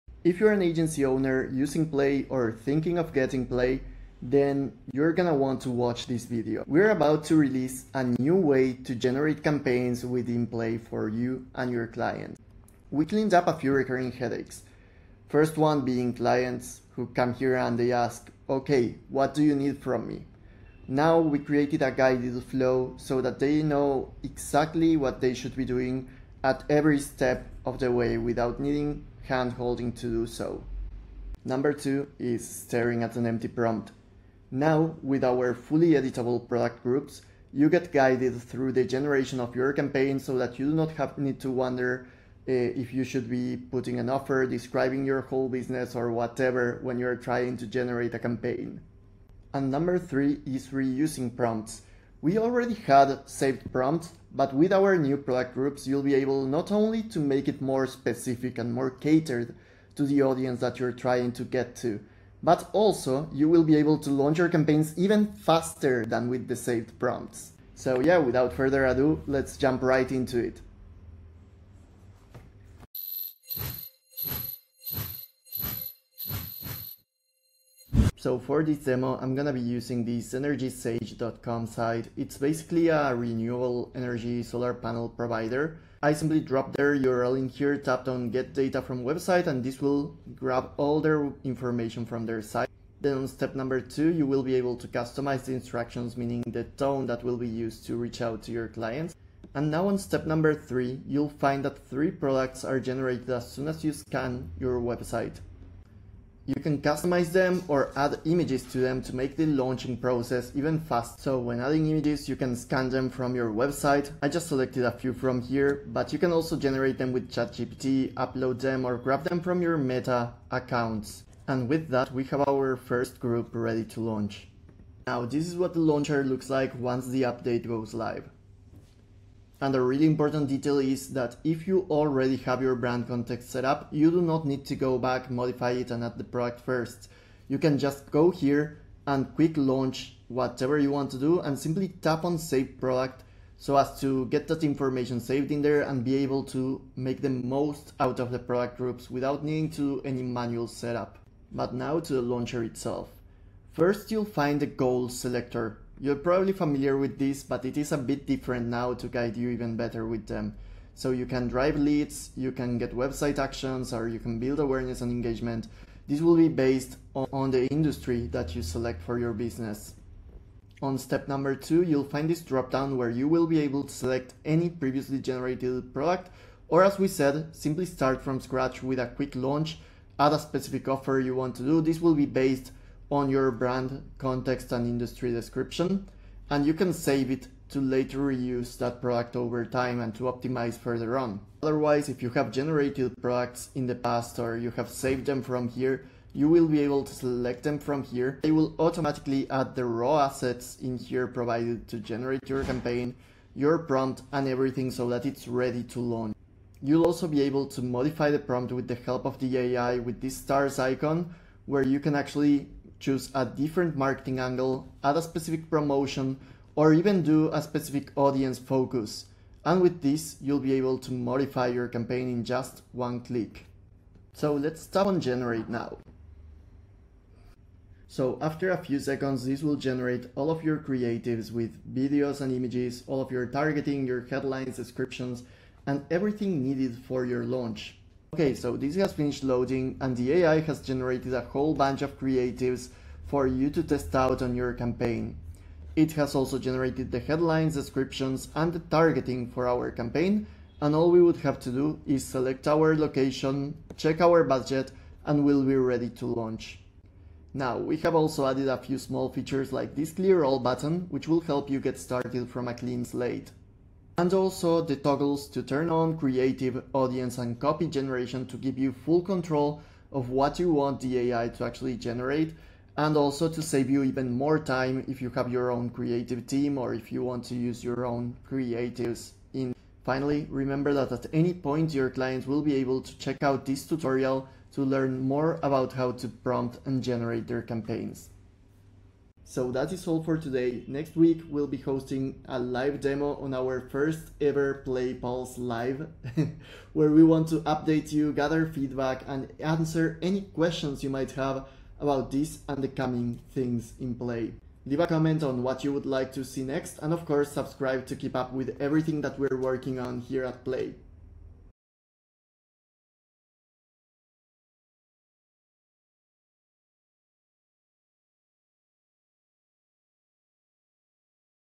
New in Plai: 1-2-3 Launcher for Faster Ads
New in Plai: a 1-2-3 launcher that helps you go from “what do you need from me?” to a live campaign in minutes—without guessing what to write.

What it does best
Clarity for your clients: a simple checklist to launch — pick goal → select product → upload unbranded images → launch.
Prompting made easy (and reusable): product groups for easy save & reuse. You describe what you know (product), let Plai do the rest.
Smarter optimization: with our new Product groups, Plai is able to learn more to take our optimizations even further with our Optimize for Me feature as well as Optimization Folders.

0:00 Intro
0:30 What this update actually fixes
1:13 Introducing the new Brand Context
2:00 Live Ad build
4:12 Other small modifications